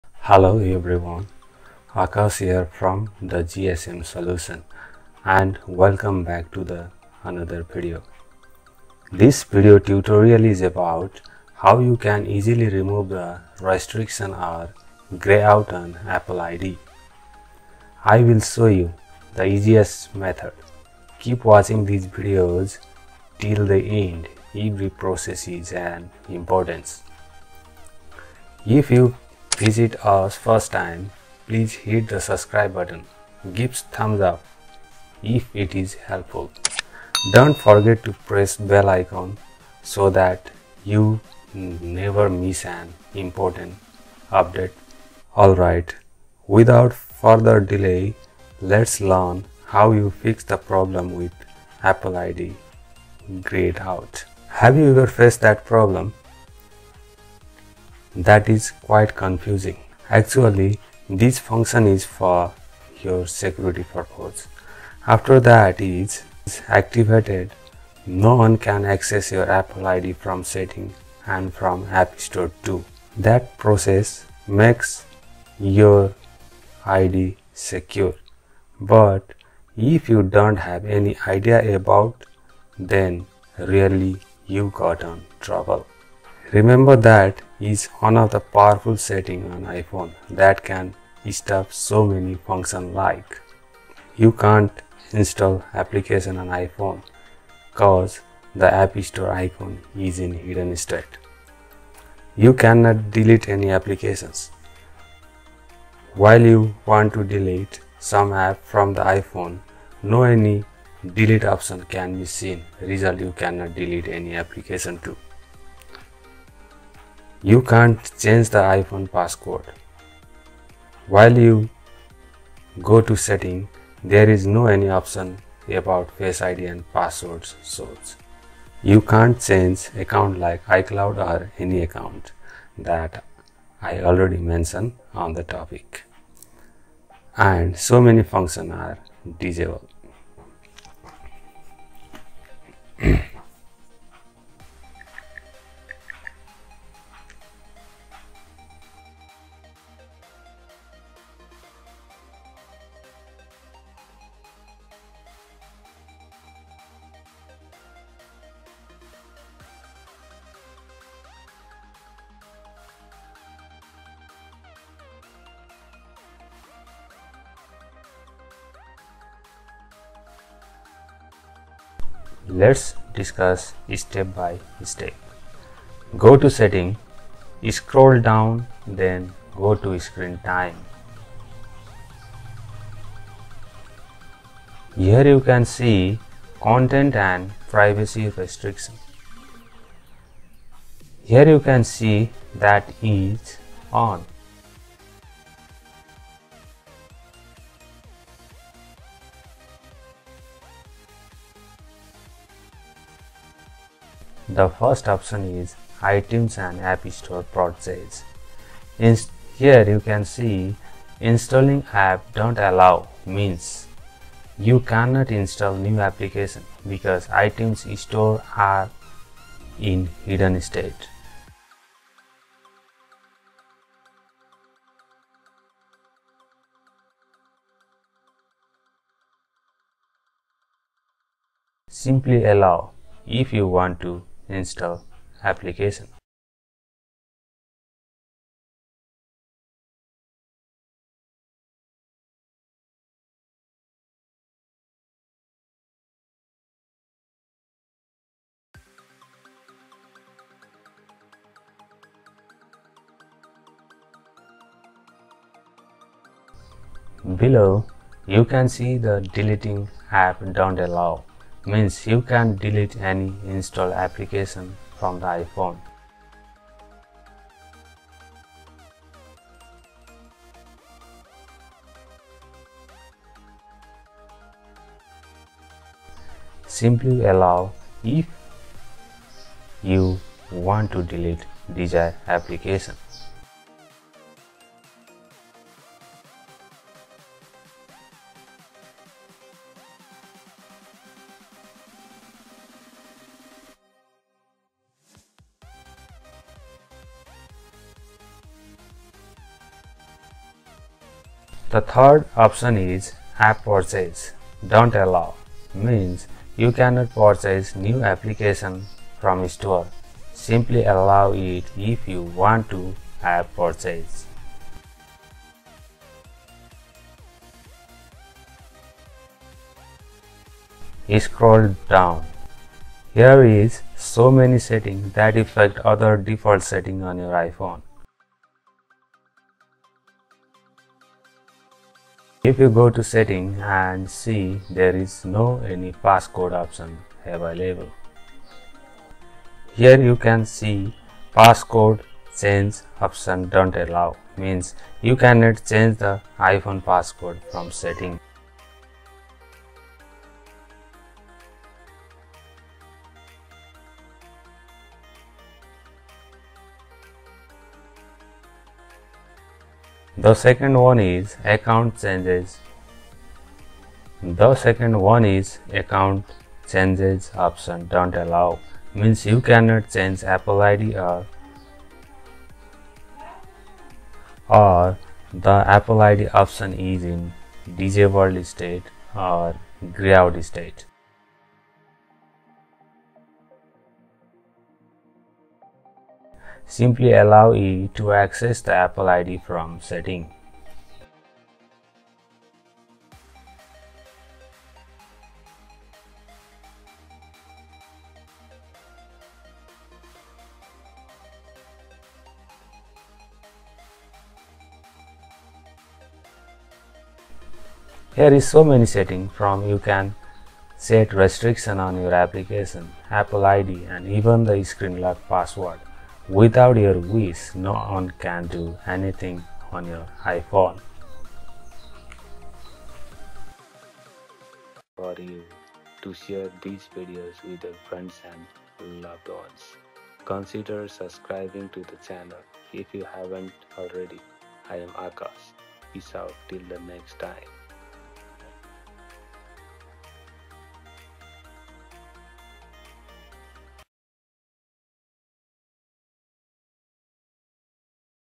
Apple id Greyed out in iPhone settings fixed! Unable to delete app from iPhone fix.
Apple id is greyed out in iphone setting on any ios and unable to delete application from iphone and also even can`t change passcode from setting.This videos tutorial is about how you can easily remove the restriction or greyed out on Apple Id.I will show you the easiest method.Keep watching this videos till the end every process is an important.

Thegsmsolution